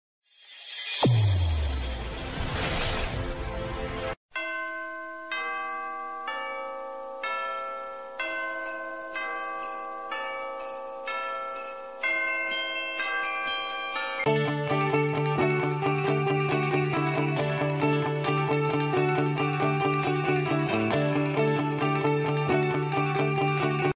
Teo gutiérrez rompió el silencio: qué dijo sobre el escándalo con las esposas de dos de sus compañe
Teo gutiérrez rompió el silencio: qué dijo sobre el escándalo con las esposas de dos de sus compañeros